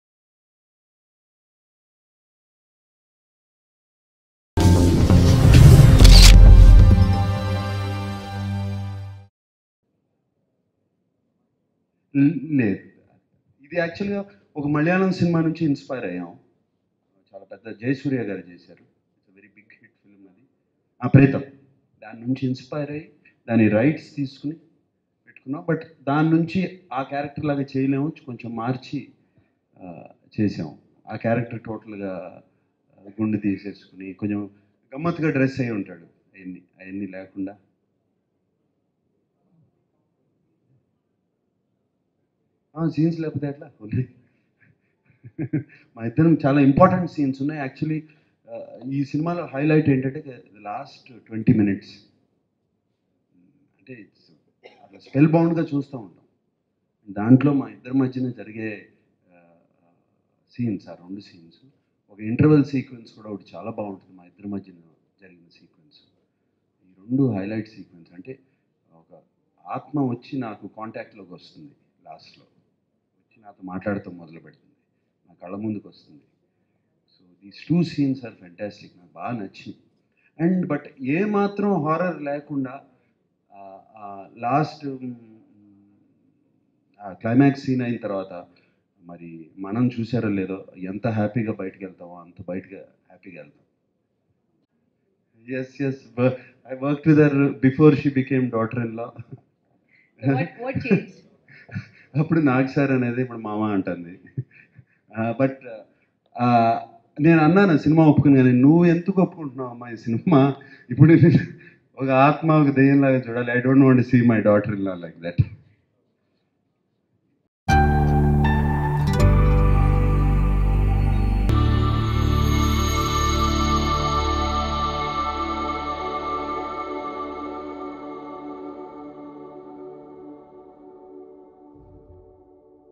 Nagarjuna Funny Comments on Samantha @Raju Gari Gadhi 2 Movie Press Meet - Filmyfocus.com
Nagarjuna Funny Comments on Samantha @Raju Gari Gadhi 2 Movie Press Meet. RajuGariGadhi2 Movie Stars Nagarjuna, Samantha, Seerath Kapoor in Lead Role. Directed by Omkar, Produced by PVP Productions, Music by S. Thaman.